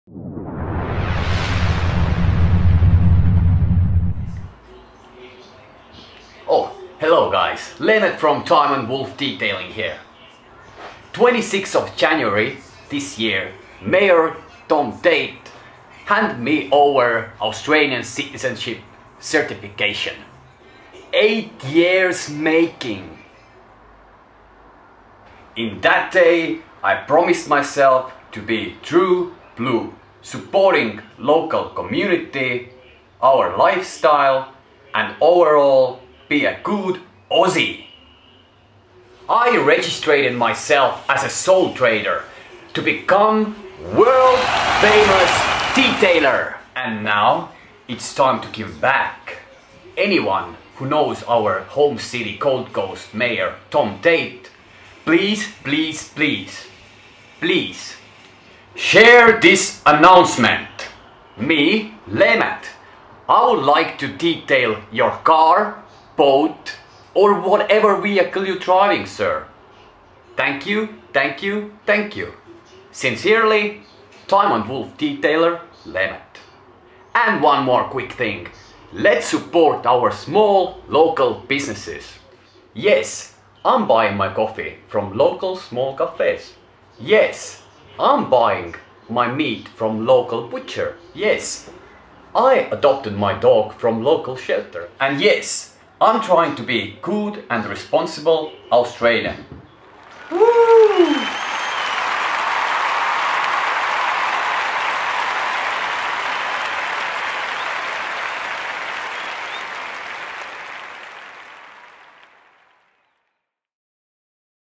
DETAILING Experiment: How to detail Mayors vehicle 🚗
Gold Coast mobile detailer calling out Major Tom Tate to detail his vehicle. Is that experiment is successful?

Supporting local community and businesses thru some humour.